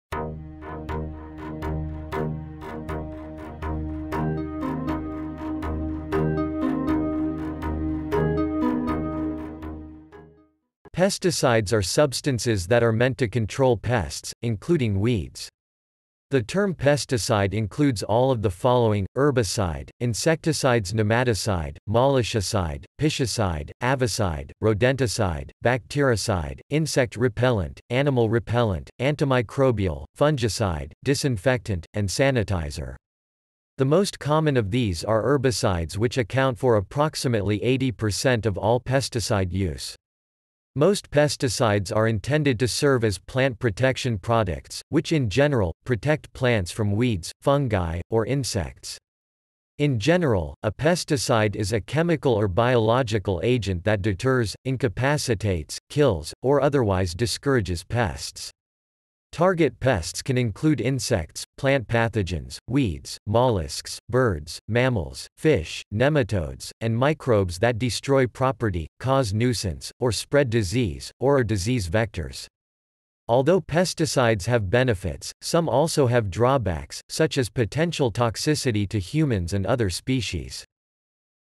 What is Pesticide? Explain Pesticide, Define Pesticide, Meaning of Pesticide
~~~ Pesticide ~~~

Title: What is Pesticide? Explain Pesticide, Define Pesticide, Meaning of Pesticide

Description: Pesticides are substances that are meant to control pests, including weeds. The term pesticide includes all of the following: herbicide, insecticides nematicide, molluscicide, piscicide, avicide, rodenticide, bactericide, insect repellent, animal repellent, antimicrobial, fungicide, disinfectant , and sanitizer. The most common of these are herbicides which account for approximately 80% of all pesticide use. Most pesticides are intended to serve as plant protection products , which in general, protect plants from weeds, fungi, or insects.  In general, a pesticide is a chemical or biological agent that deters, incapacitates, kills, or otherwise discourages pests. Target pests can include insects, plant pathogens, weeds, molluscs, birds, mammals, fish, nematodes , and microbes that destroy property, cause nuisance, or spread disease, or are disease vectors. Although pesticides have benefits, some also have drawbacks, such as potential toxicity to humans and other species.